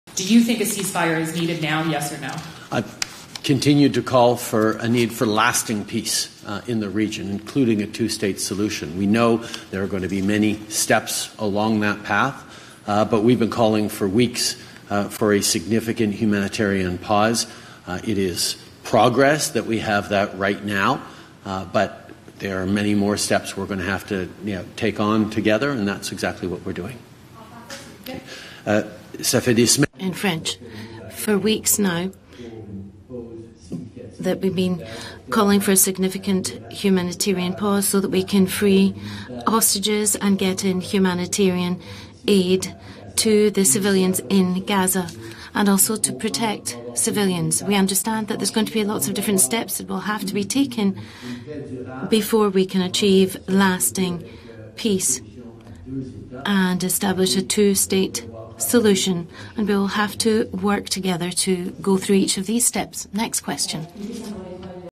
Hamas hostage deal 'progress' but long term peace needs 'many more steps'! Trudeau debates
Prime Minister Justin Trudeau remained unmoved on Friday in rejecting demands for Canada to call for a full ceasefire in the Israel-Hamas war, but said a deal this week to temporarily halt hostilities so some hostages can be released is a sign of progress.
"I continue to call for a need for lasting peace in the region, including a two-state solution," Trudeau said at a news conference in St. John's, N.L.

"We've been calling for weeks for a significant humanitarian pause. It is progress that we have that right now. But there are many more steps we're going to have to take on together."

Hamas, which Canada deems a terrorist organization, has so far freed 24 people, including 13 Israeli women and children, 10 people from Thailand and one person from the Philippines.

Israel confirmed on Friday that it released 39 Palestinian prisoners in line with the truce deal.

Hamas captured about 240 hostages when its militants attacked Israel on Oct. 7, killing about 1,200 people, including hundreds of civilians.

An official at Global Affairs Canada confirmed to the Senate foreign affairs committee this week that one Canadian woman is still missing.

The pause in fighting is a respite for millions displaced or injured amid Israel's retaliation campaign in the Hamas-controlled Gaza Strip, where local health authorities say 13,300 Palestinians have been killed and another 6,000 people are missing.

Trudeau did not specify Friday why he does not support a ceasefire. Last month, Defence Minister Bill Blair said he did not expect that Hamas would respect one, saying Oct. 24 that the organization must "be eliminated as a threat, not just to Israel, but to the world."

On Parliament Hill, groups supporting Palestinians are continuing to call for a ceasefire, saying there needs to be time to build housing for the 1.7 million people the United Nations says have been displaced in Gaza.

Fahamia Koudra, a board member of Human Concern International, said her colleagues in the region have seen an uptick in deaths from dehydration and vaccine-preventable deaths.

"Winter is coming and there is no sanitation or water," she said at a news conference Friday morning.

"A four-day pause is not even enough to retrieve bodies from the rubble," she said. "Without a ceasefire, mass casualties will exponentially increase without the desperately needed supplies and (with) devastated infrastructure."

On Thursday, Ottawa resident Hany Elbatnigi recounted horrifying scenes in Gaza, which he left on Nov. 7, at a news conference on Parliament Hill.

"I won the lottery of life. I was one of the lucky ones to leave for safety to Cairo," he said.

"I left behind the smell of death, the horrifying sounds of explosion and the sight of dogs eating the flesh of the dead bodies."

Elbatnigi said his family left home after Israeli military airstrikes bombarded his neighbourhood. They stayed in homes crammed with dozens of people for days at a time, progressively moving south toward the Egyptian border.

He said 71 members of his extended family have been killed in Gaza, with him learning Wednesday that his niece and her two children died.

"Too many of them were barely taller than my waist," he said.

Elbatnigi lamented Canada's strident support of Israel during its war, and the communication from consular officials.

"There was very little information provided and even less help," he said. "Instead, there were many public statements that Canada supported Israeli attacks while my family and I were still in harm's way."

"I cannot help them, but the least I can do is to ask my government to call for a ceasefire. The people of Gaza deserve to be lucky like me."

European Council President Charles Michel reiterated Europe's support for a two-state solution in the Middle East before formal talks got underway this morning at the EU-Canada Summit in Newfoundland.
Michel told reporters in St. John's the leaders will also discuss Israel's right to defend itself within the bounds of international law and the humanitarian crisis in Gaza.

He says the meeting will include conversations about how to best support Ukraine and sanction Russia.

Prime Minister Justin Trudeau did not directly address the war between Israel and Hamas nor the Russian invasion of Ukraine, but he acknowledged the summit is happening as the world faces "extremely complex times."